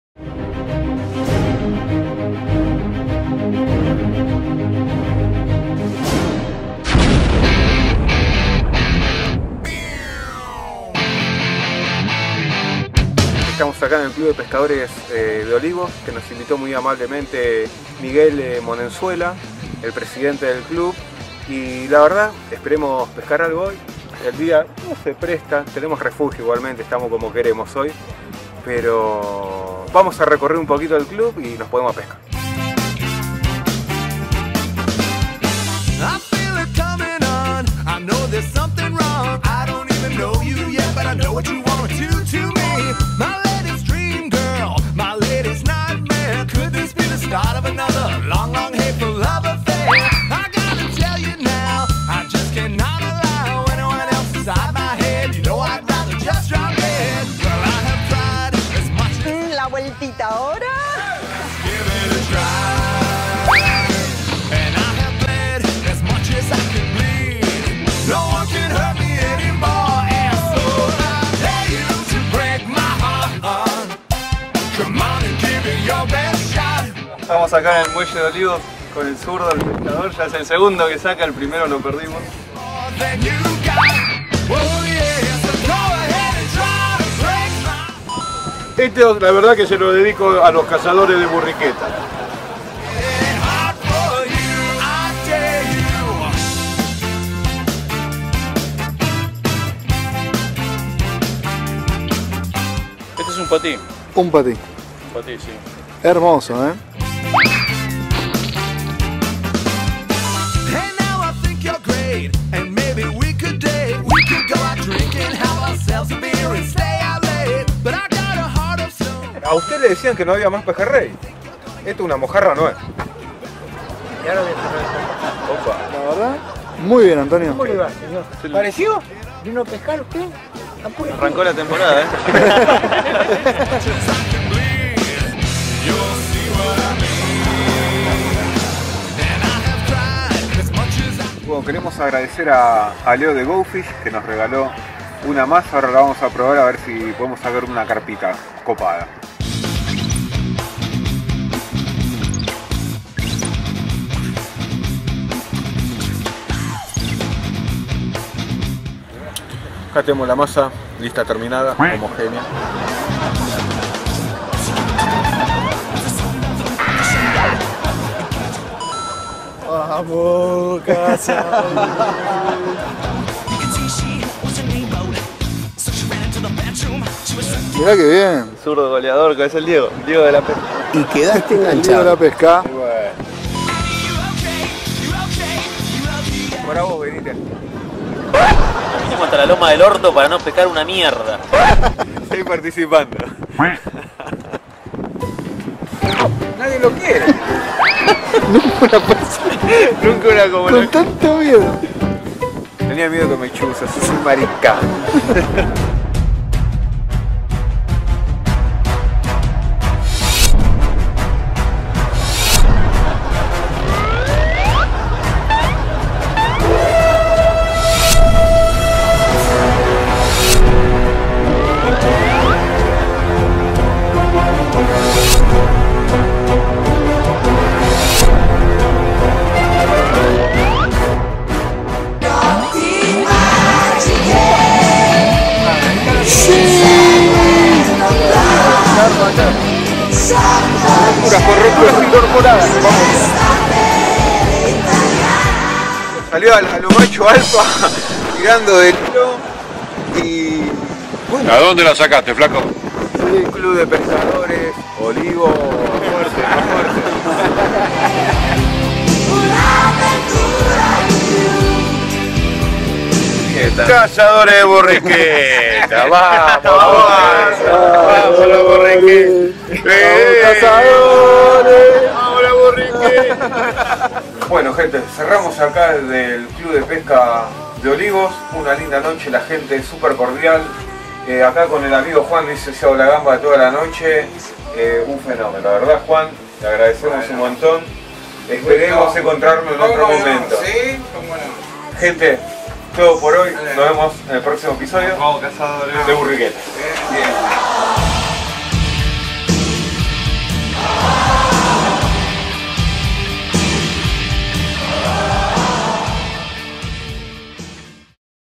Cazadores de Burriquetas en Club de pescadores Olivos - Pesca de carpa record al final(?)
Compartimos un nuevo capitulo de los "Cazadores De Burriquetas". En esta ocasión, un día en el club de pescadores de Olivos"